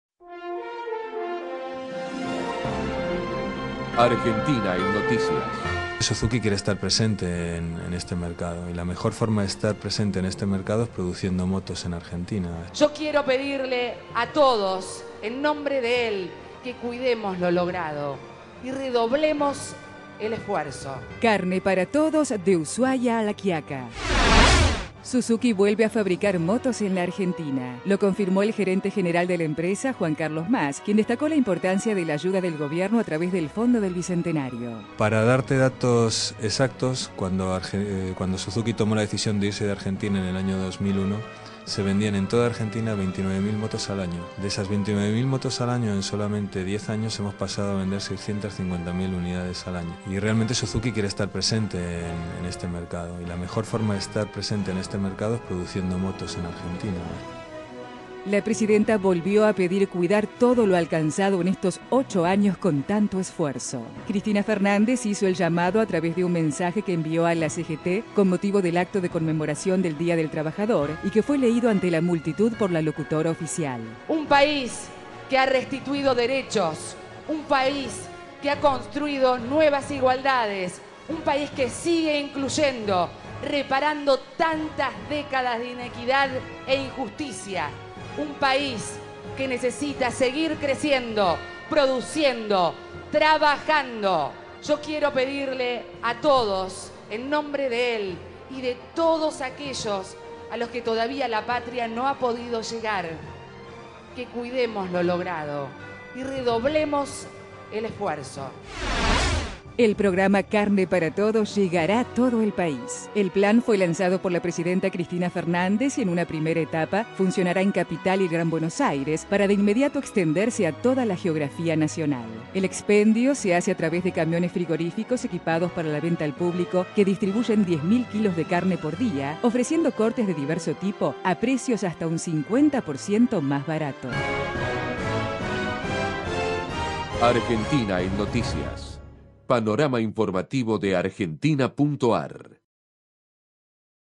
AEN 02-5-2011 11 hs: Suzuki motos argentina - Acto dia del trabajador - Carne para todos
SUZUKI VUELVE A FABRICAR MOTOS EN LA ARGENTINA
LA PRESIDENTA CRISTINA FERNANDEZ ENVIO UN MENSAJE A LOS TRABAJADORES QUE SE LEYO EN EL ACTO CENTRAL CONVOCADO POR LA CGT
EL PROGRAMA "CARNE PARA TODOS" SE EXTENDERA A A TODO EL PAIS